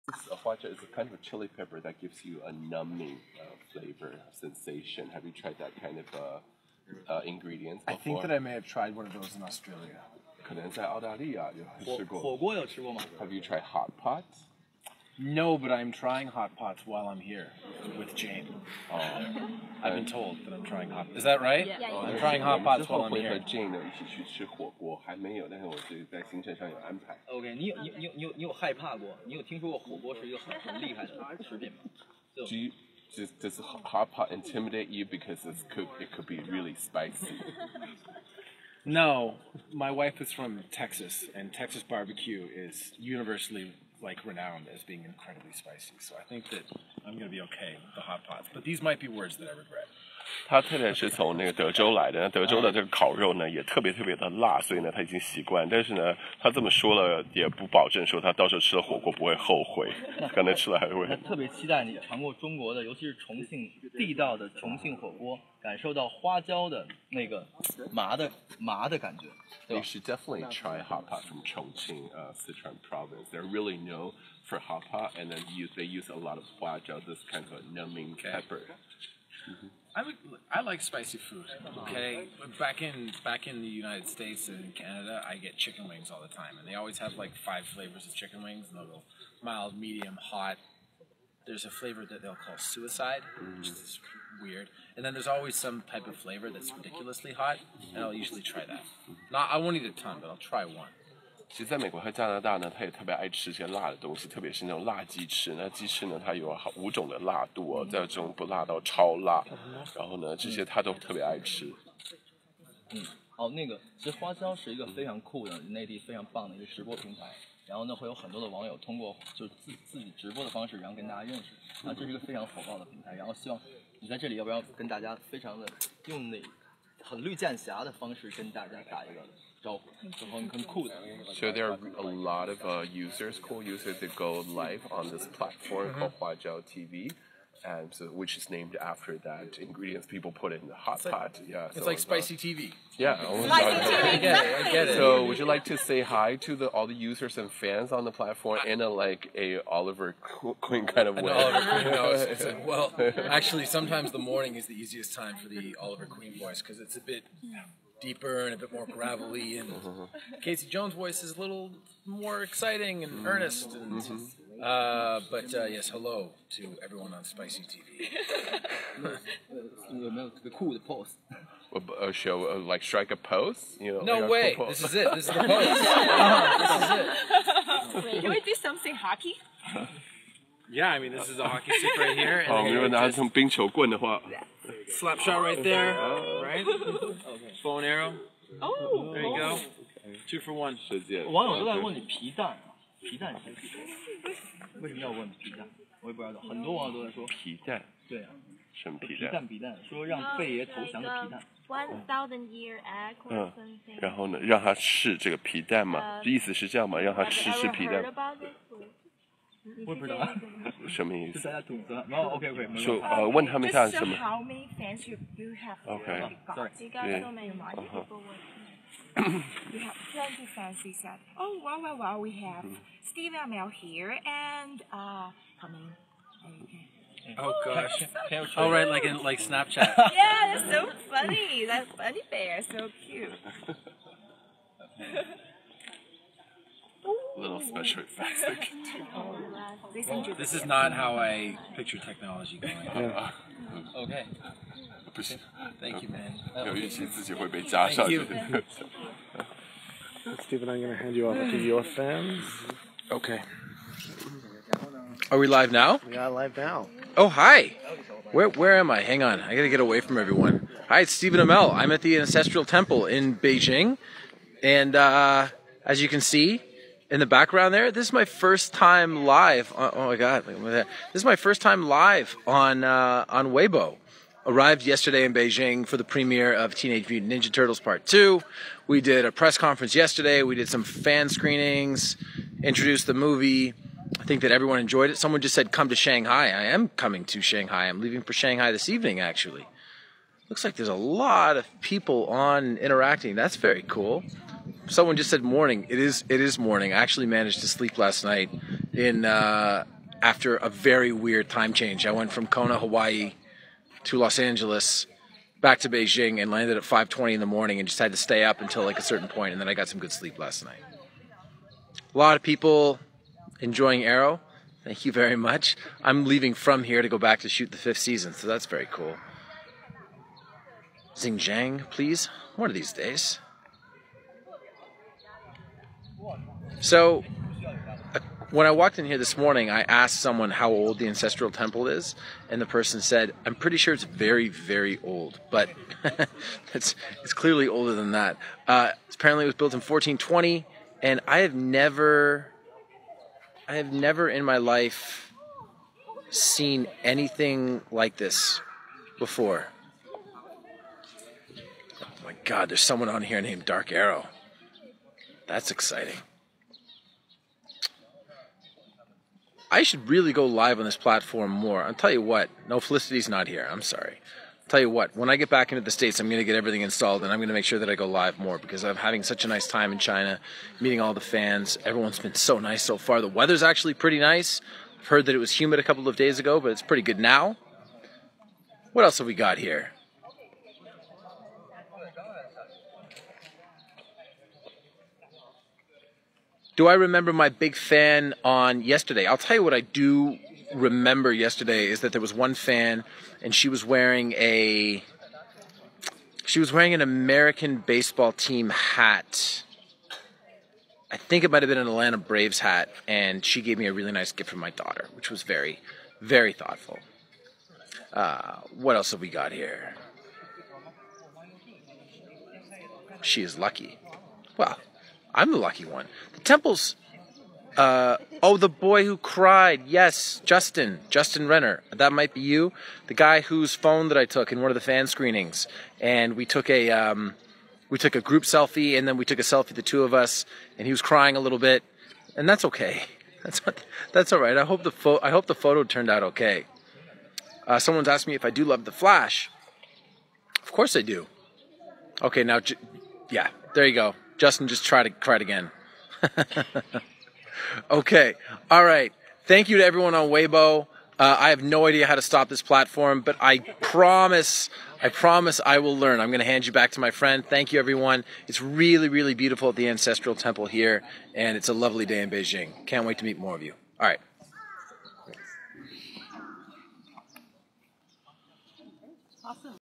Stephen Amell's live video from the Ancestral Temple in Beijing
Stephen Amell is in China to promote his movie "Teenage Mutant Ninja Turtles: Out of the shadow"